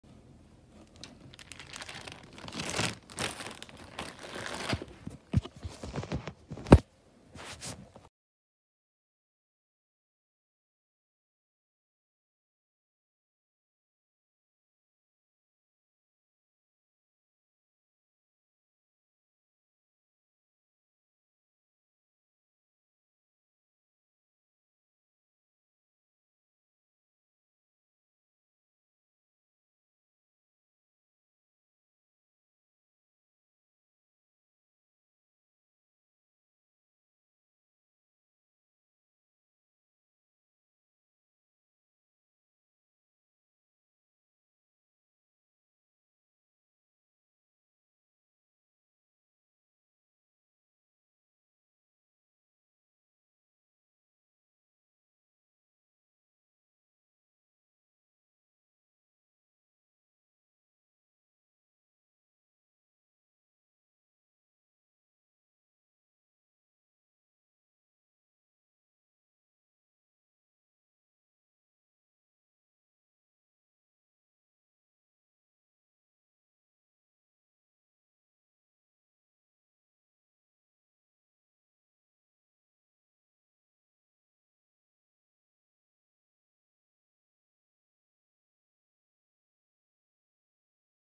I Made This On An Airplane…
As I said in my last video’s description, I would recreate the volleyball scene on an airplane! And there it is.